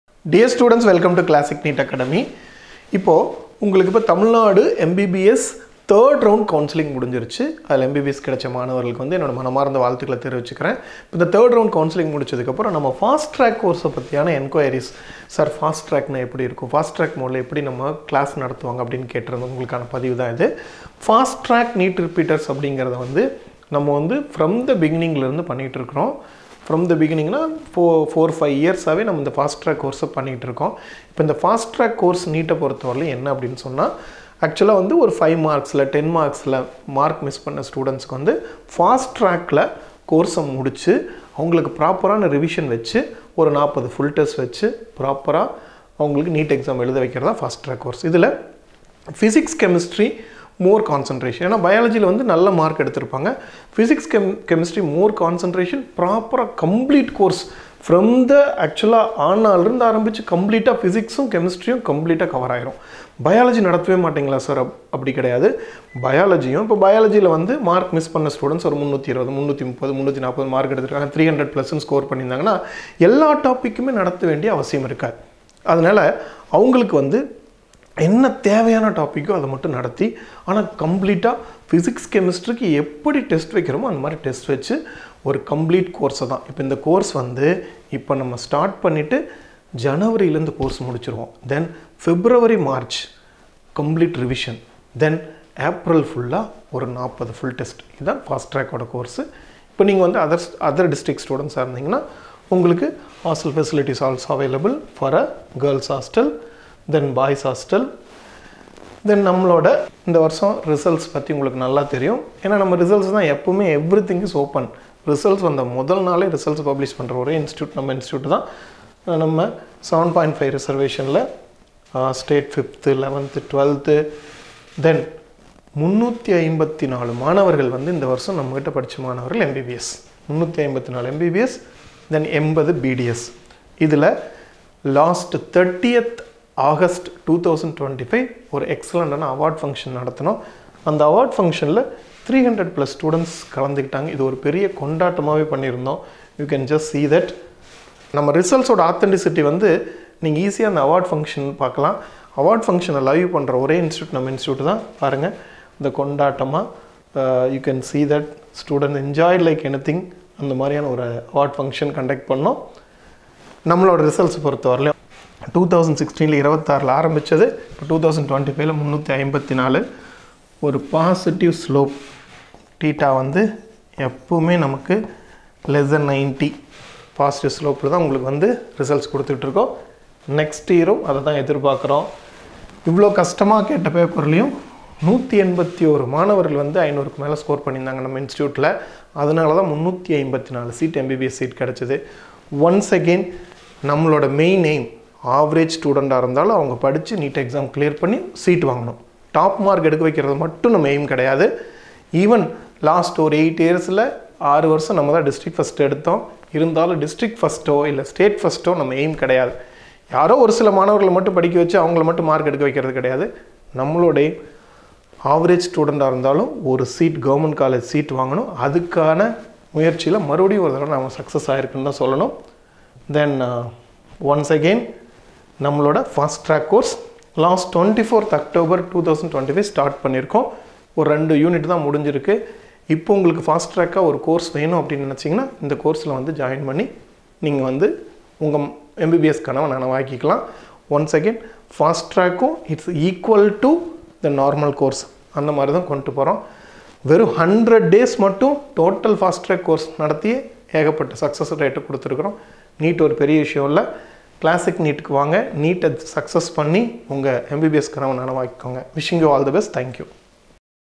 Join the Fast Track NEET Repeaters Program 2026 | Limited Seats Available | Classic NEET Academy
Boost your NEET 2026 preparation with Classic NEET Academy’s Fast Track Repeaters Course!
This program is specially designed for repeaters aiming to achieve top scores in NEET 2026 within a short time. Our expert faculty, focused study plan, and proven results will help you turn your medical dream into reality.

✅ Intensive revision & test series
✅ Concept clarity through expert guidance
✅ Personalized mentoring & doubt sessions
✅ Hostel facilities available